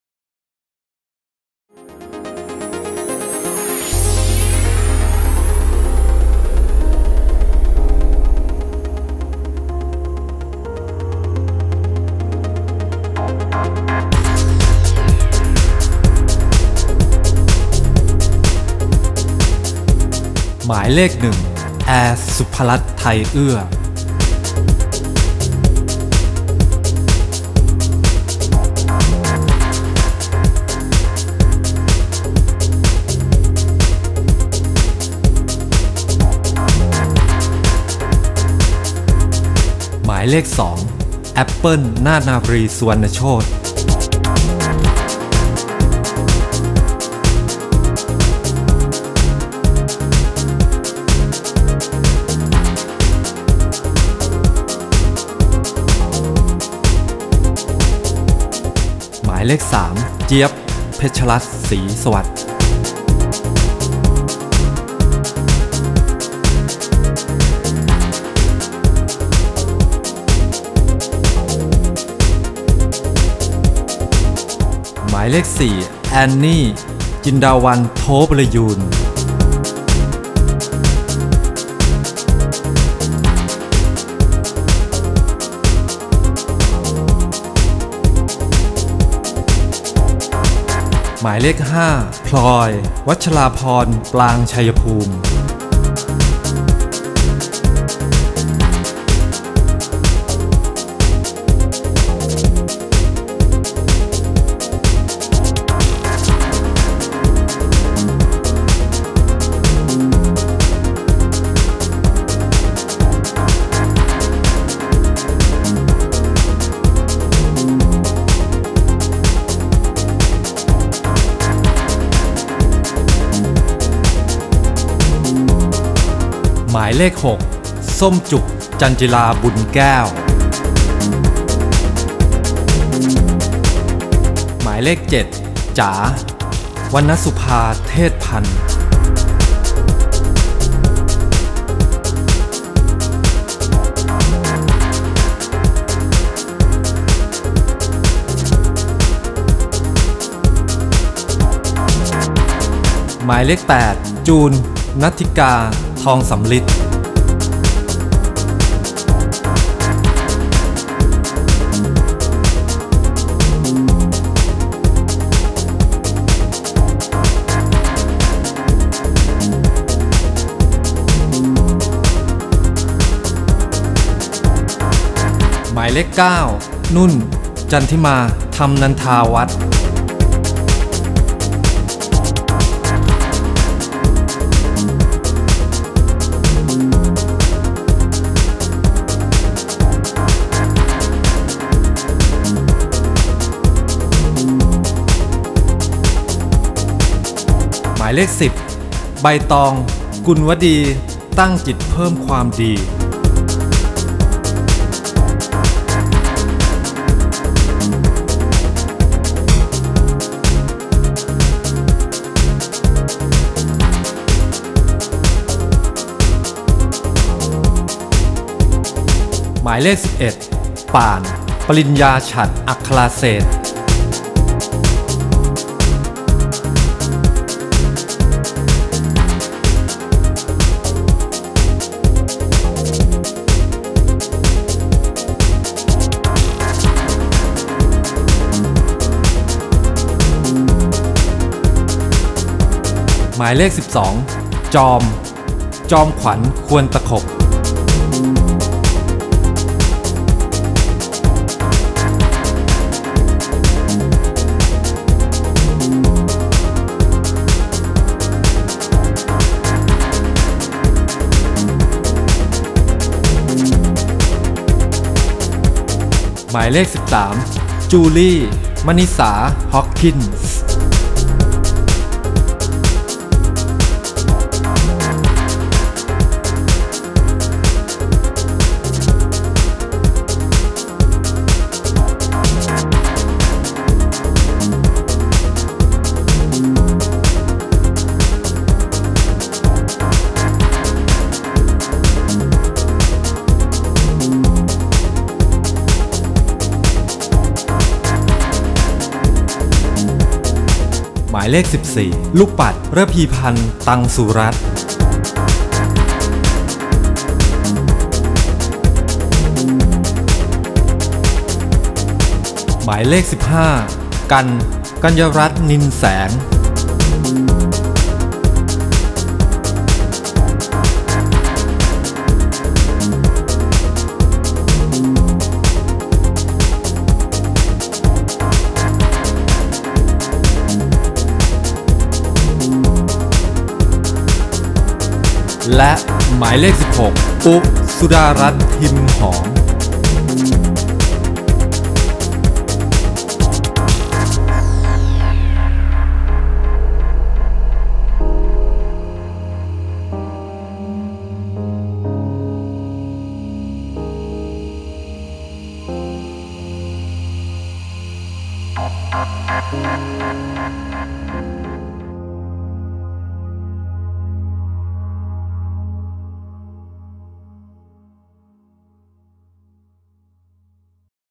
Miss Beauty Queen Thailand 2012 VTR ผู้เข้าประกวด 16 คน
โฉมหน้า 16 คนสุดท้าย มิสบิวตี้ควีนไทยแลนด์

ประกวดวันที่ 10 ธ.ค. 2555  เวลา 17.00 น. เป็นต้นไป  ณ ศาลาประชาคม ริมน้ำบ้านโป่ง

สเตดิแคม / คาเมรา โอเปอเรเตอร์ / ตัดต่อ   ยุทธนา เสนีวงศ์
ฟิล์มแคนดู โปรดักชั่น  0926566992